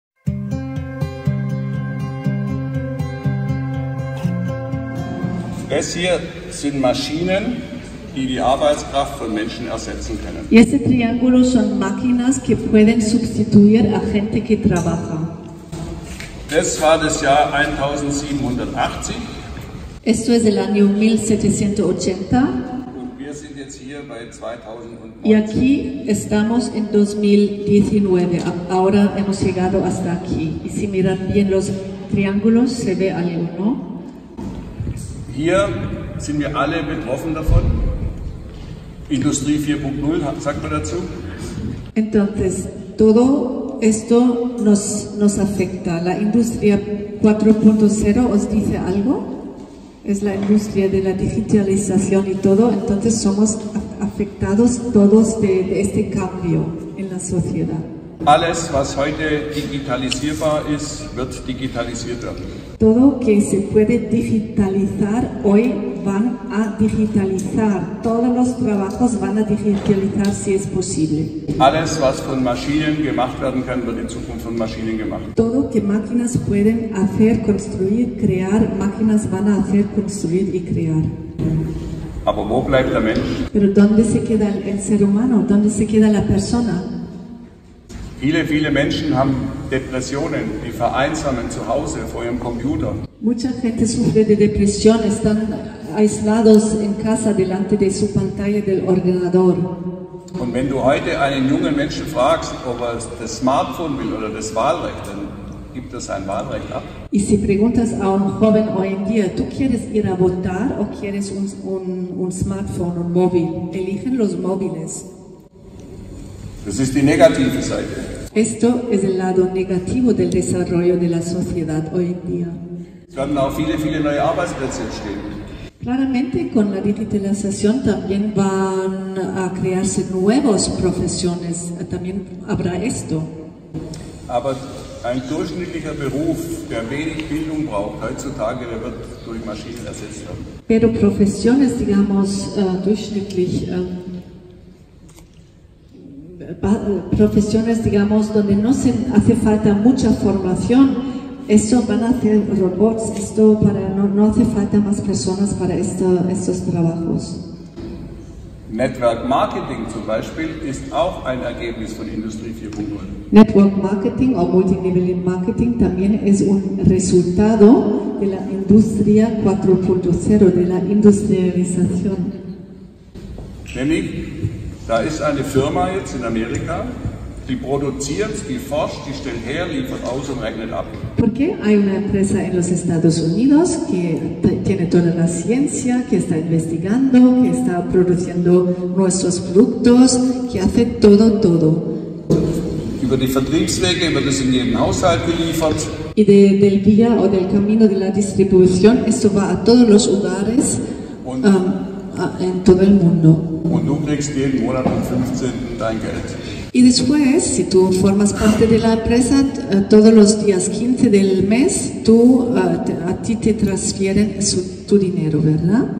Team - Valencia 2019 Workshop Industrie 4.0 Einkommen
Aktivtage in Valencia Nov 2019
Chancen für Spanien. Persönlichkeitsentwicklung Einkommensquellen der Zukunft mit
Lourdes Simo, Regine Kraft, Nil & Mathias Müller in Deutsch / Spanisch